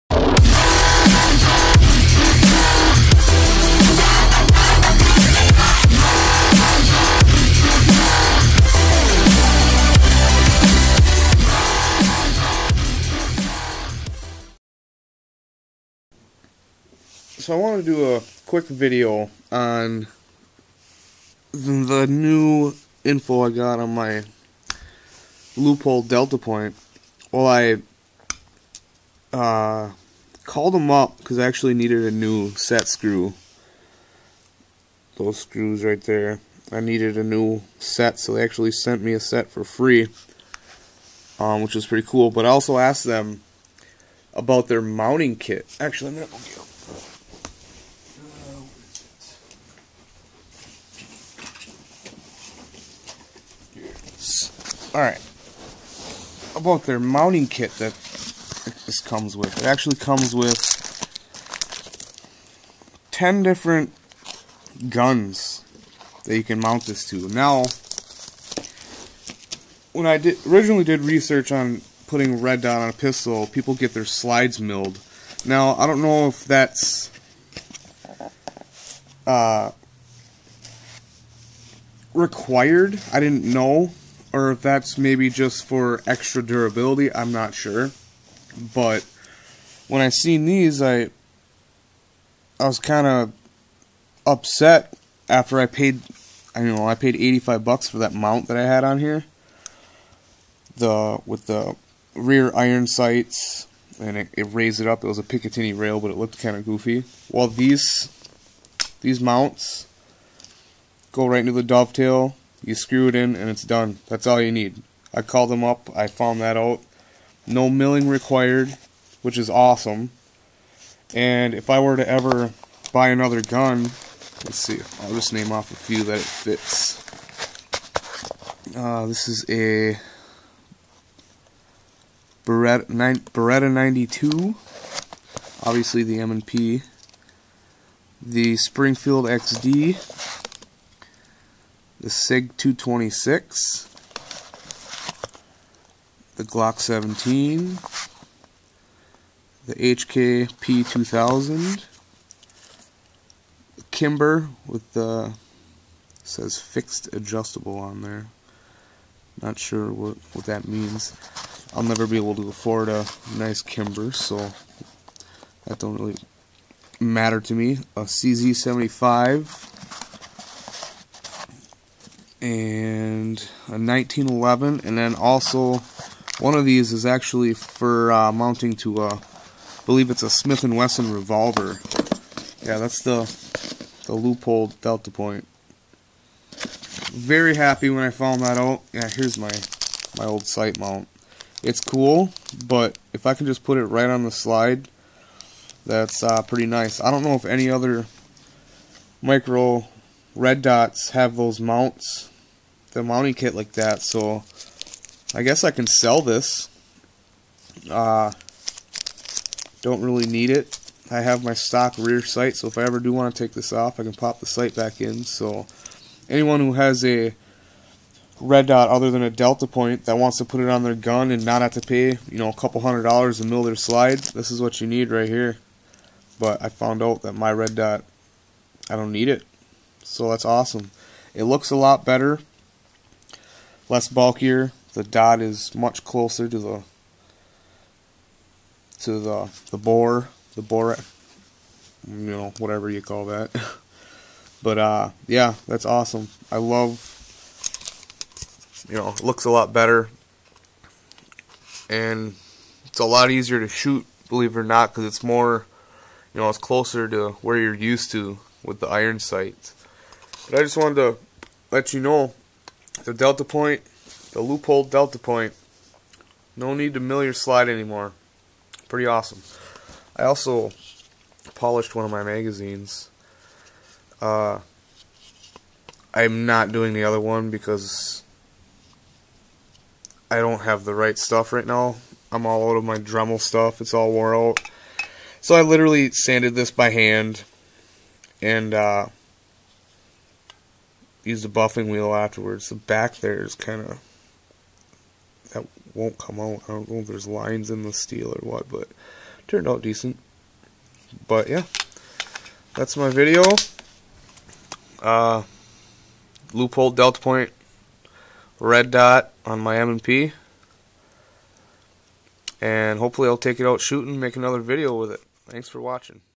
M&P: Delta Point Red Dot
Here is my M&P 9 with the Leupold Delta Point red dot. One version of this RDS comes with mounting kits for 10 different pistols, and no slide work is needed. It is raised off the slide maybe a 1/16 o